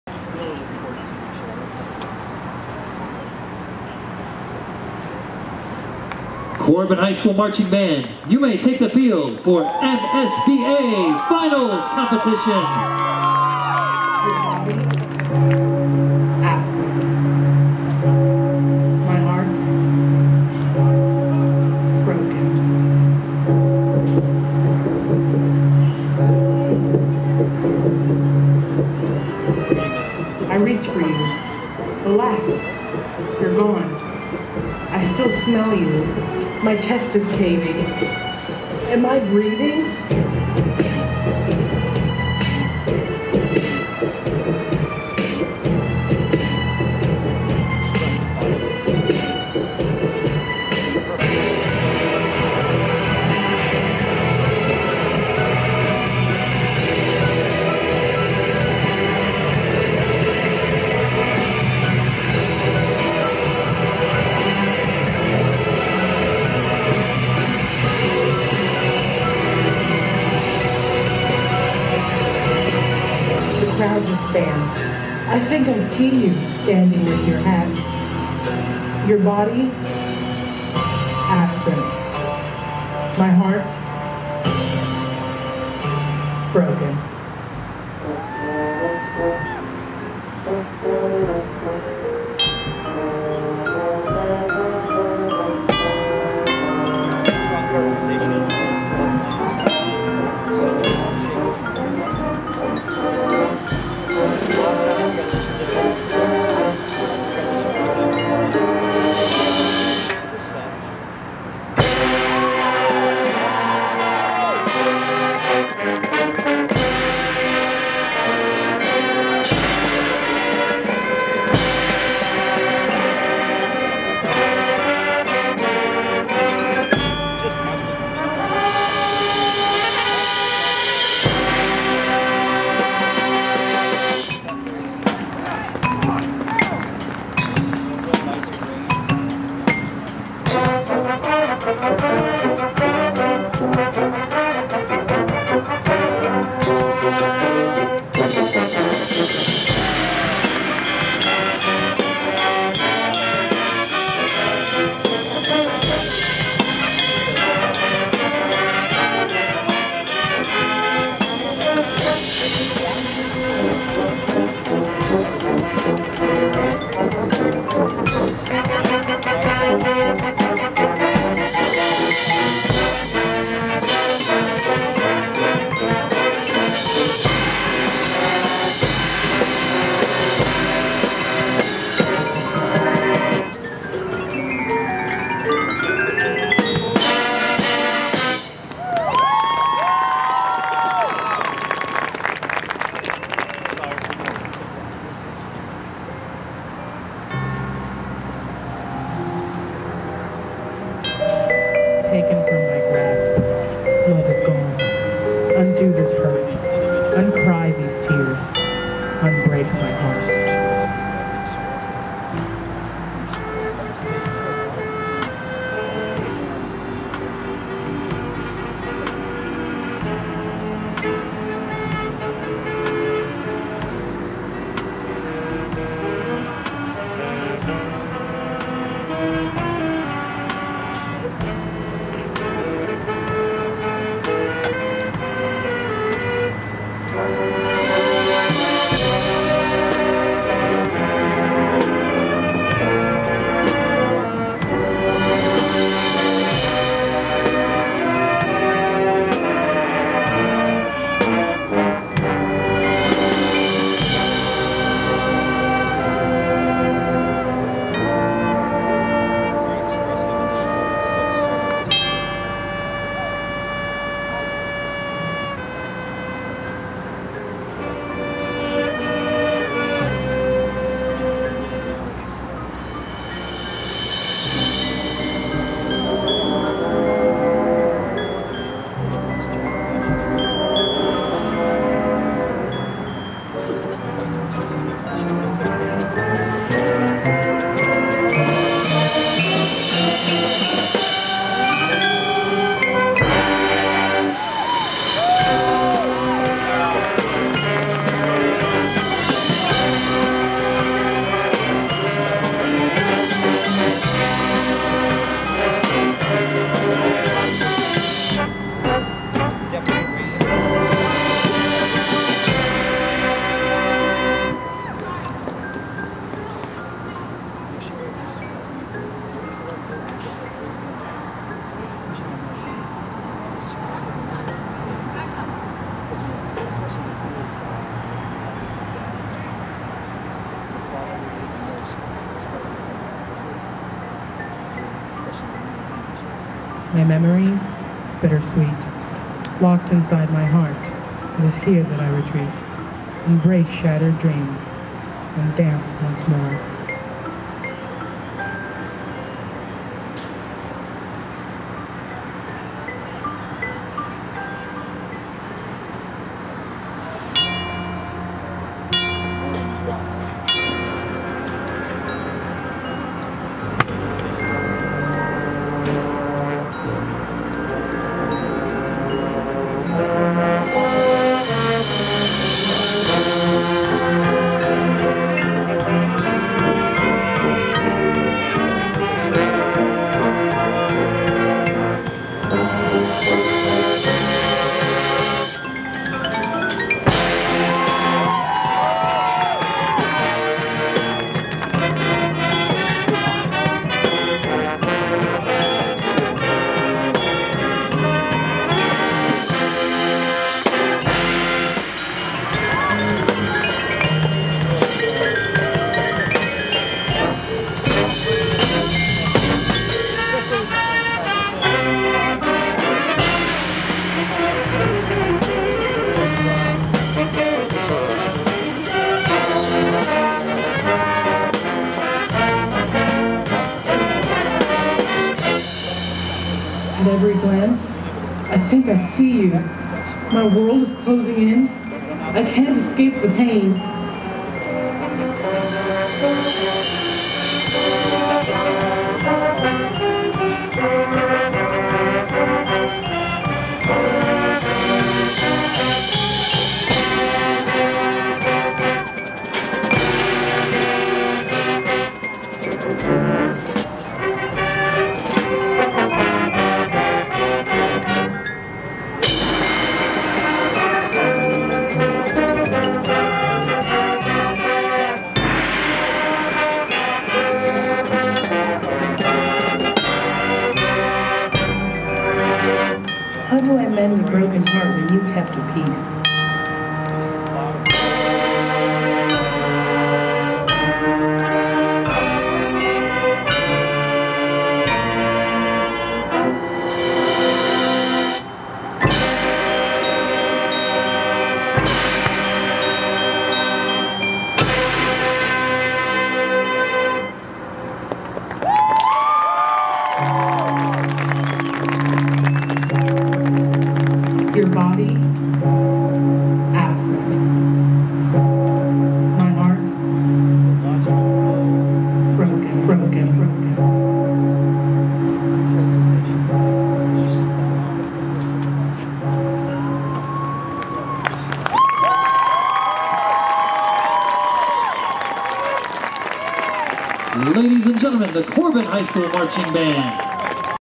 9 Corbin
Corbin HS Marching Band MSBA 2015 Class A Championships